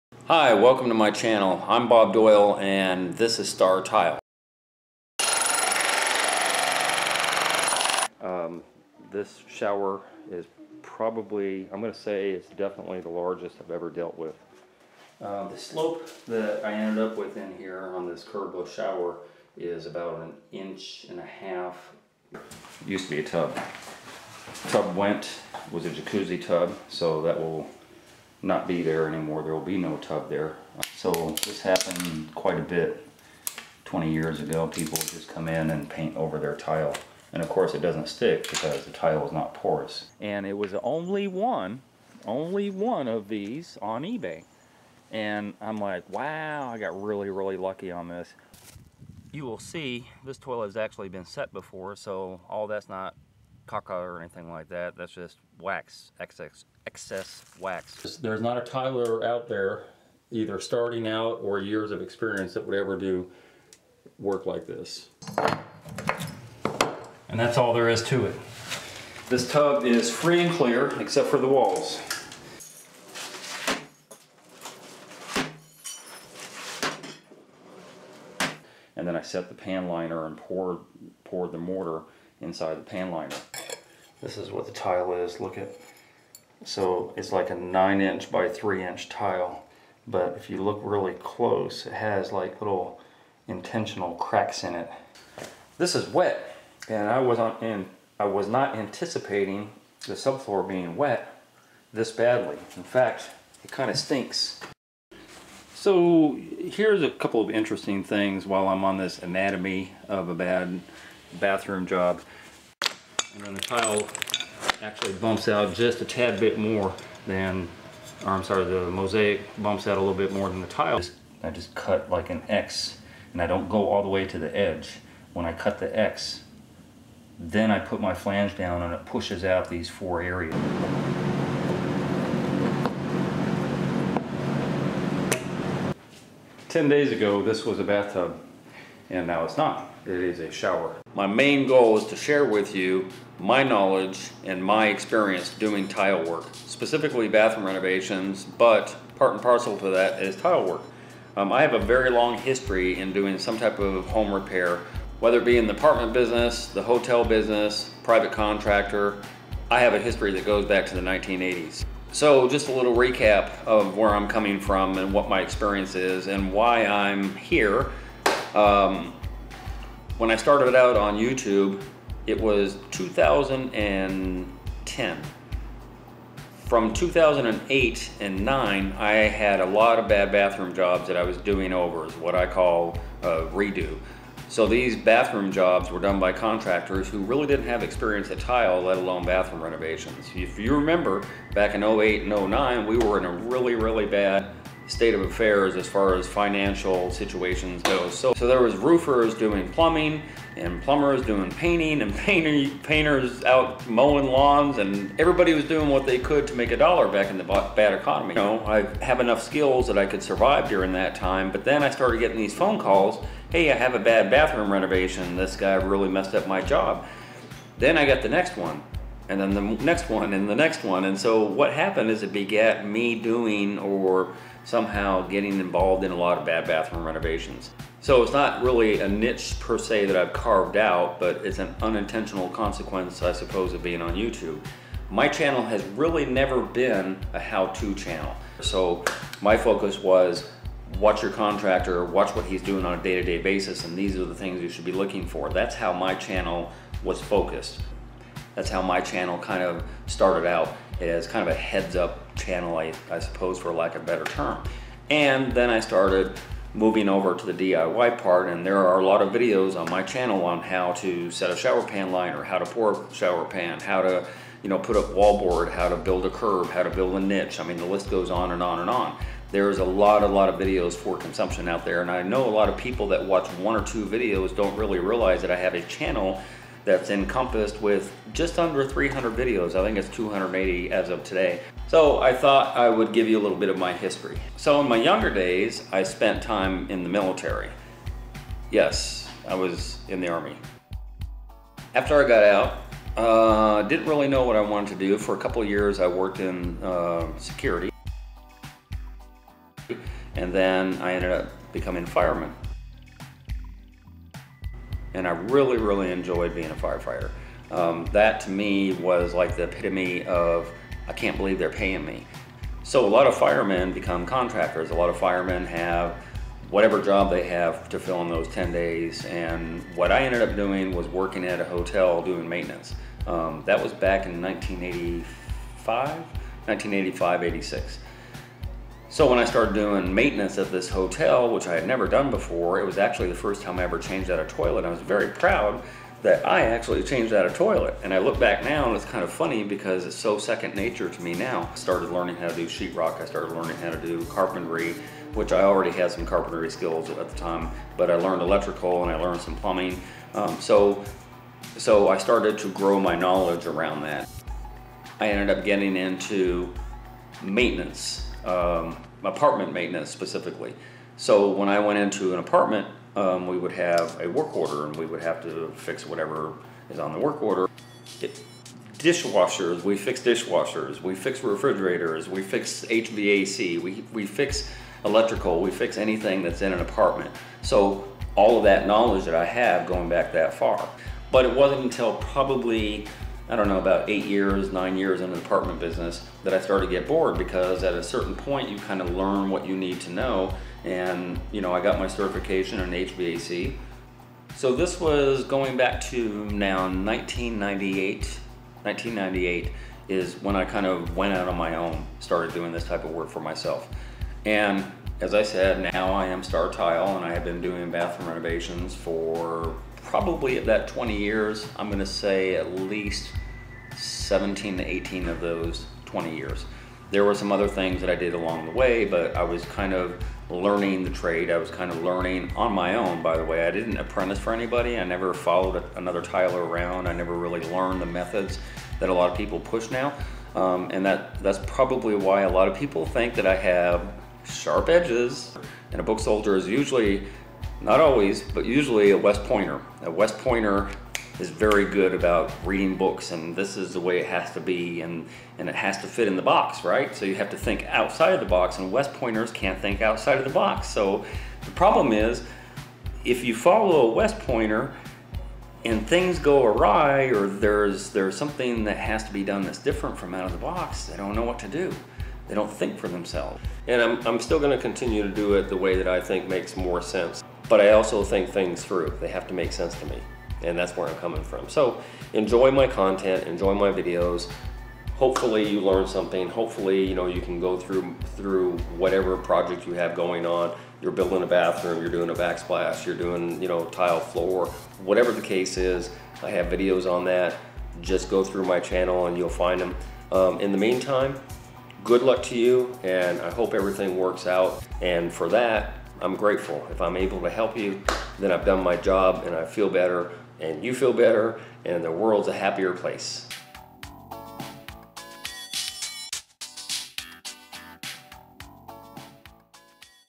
My Intro Video...FINALLY !
I've never had a "proper" channel trailer, no one has ANY idea who I am or where I gained my knowledge. So here it is finally ! Over 33 years of experience in some kind of home repair...minus my days as a mason tender, laborer with a construction company, and a short stint with a tree company, AND I'm a certified Home Inspector 2011...WHEW ! I've done a lot !